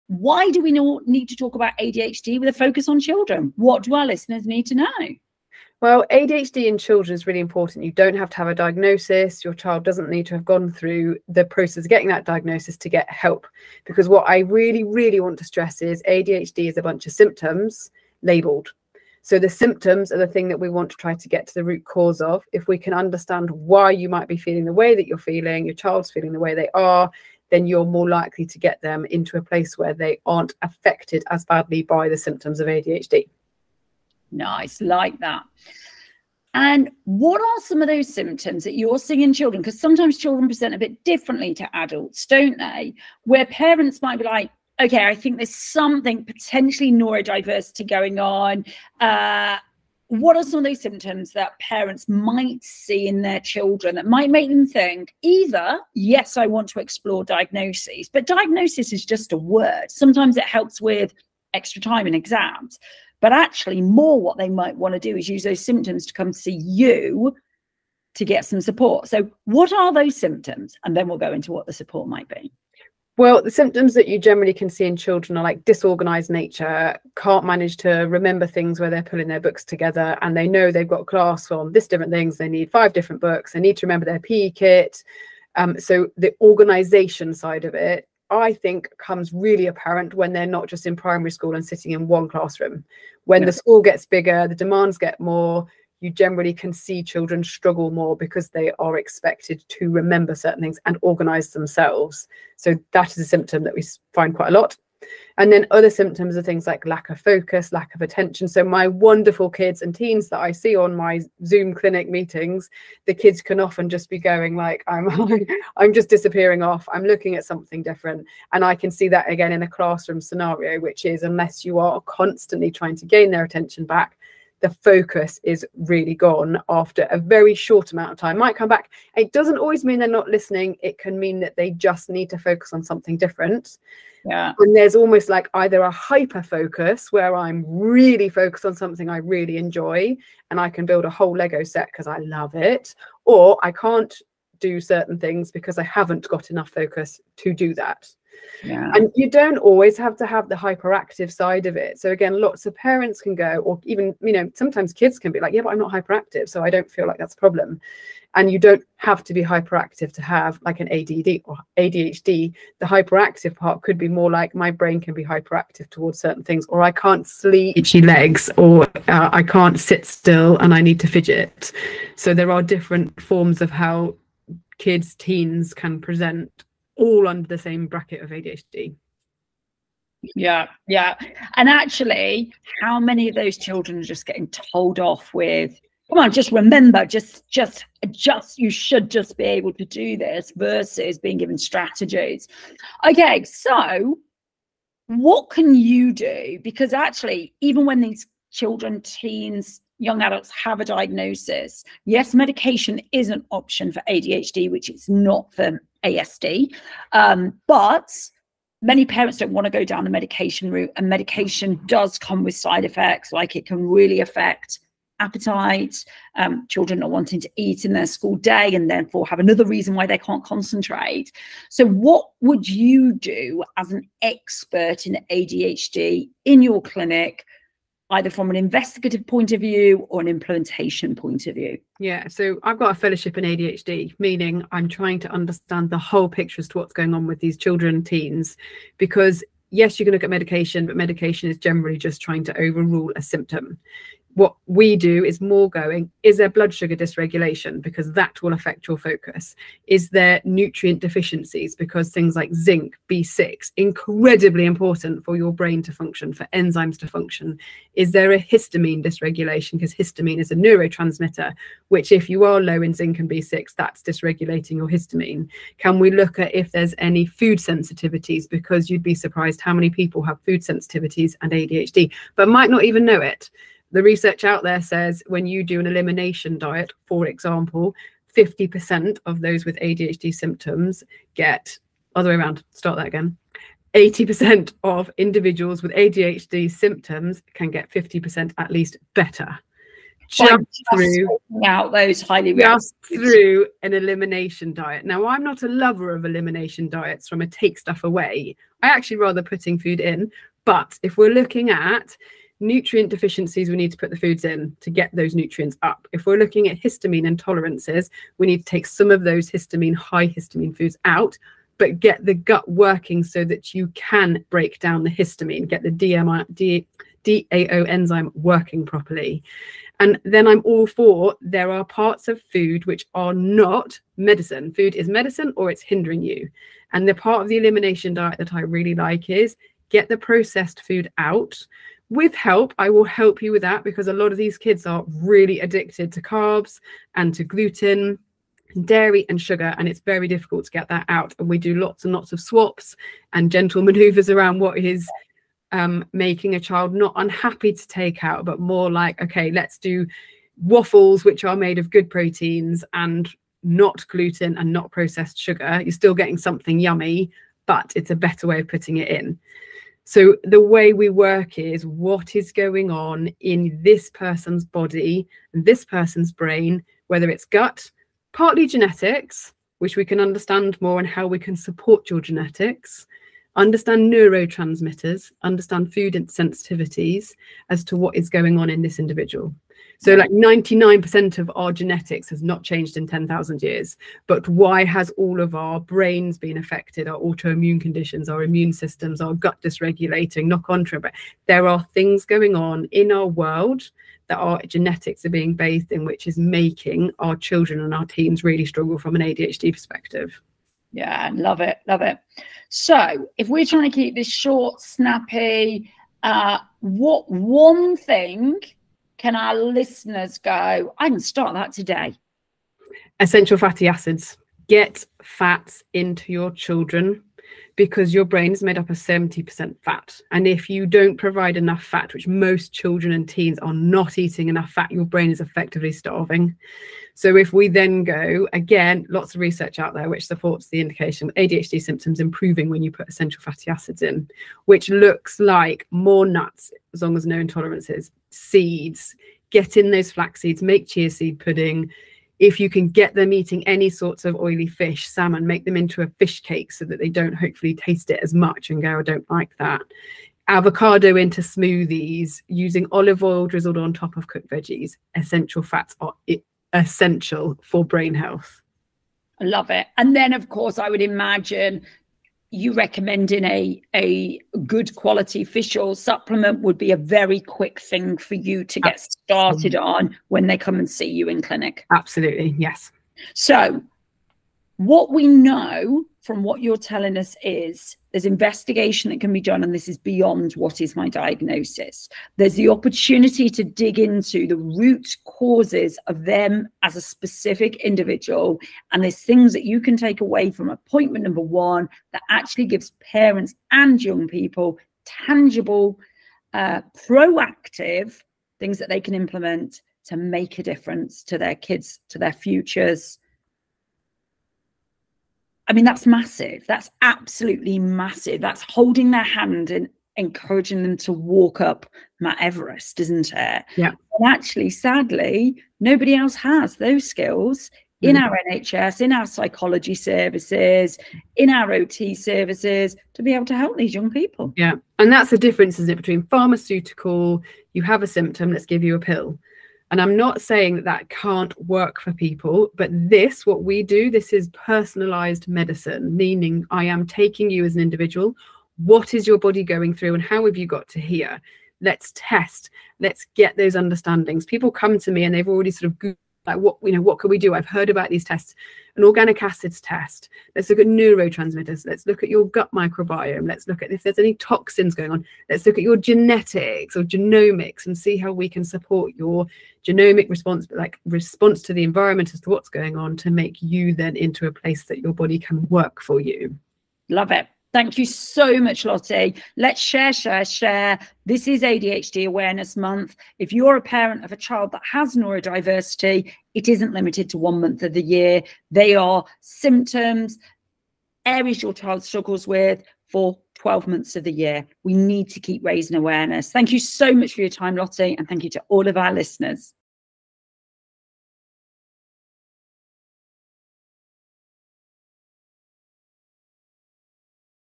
Alternatives to ADHD Medication for your child: How could Functional Medicine Help ADHD?
ADHD isn't just a label but a spectrum of symptoms that vary in intensity. Is your child facing ADHD challenges? Being labelled as a 'naughty' child at school because they can't sit still?

No diagnosis? No problem. You're in the right place.

ADHD doesn't only mean hyperactivity; it encompasses difficulties in focusing, memory, and responsibility management - just to name a few.

Here, we stand by the belief that medication isn't the sole answer and that ADHD doesn't need to be "cured" but it can be managed.

Our goal is to empower your child to reach their fullest potential, tackling not just attention struggles but also exploring underlying factors that could be compounding the symptoms like food sensitivities and blood sugar imbalances.

Lottie has a fellowship in ADHD and has plenty of ways to dig down into your child health and figure out how best they can be supported - diagnosis or not!

And while this video is talking about children, the same can be applied to adults too. Come and speak with us.